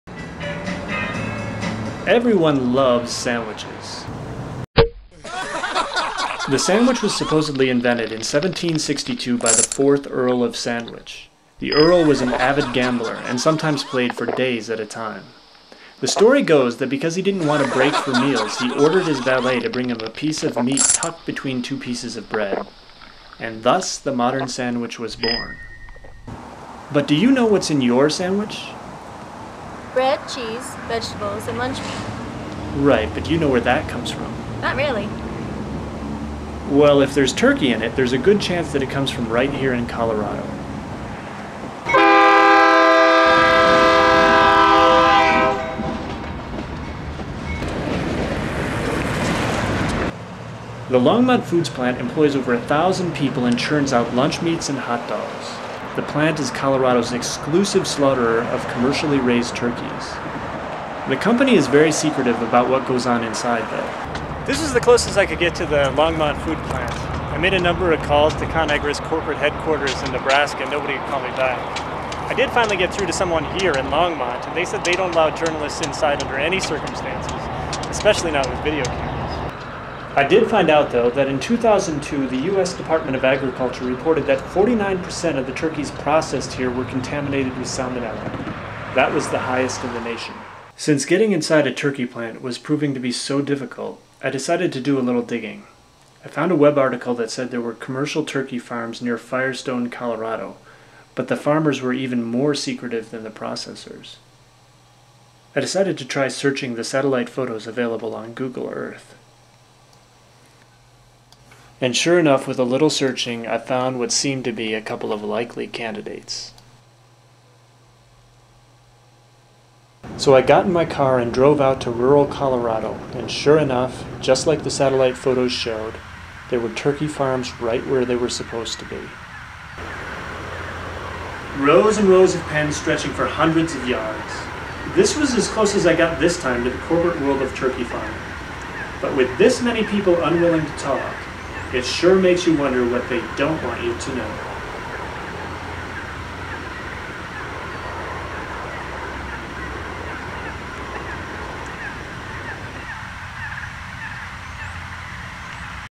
The Earl of Sandwich
An all-around look at one of the world's favorite foods - from its beginnings up to modern production.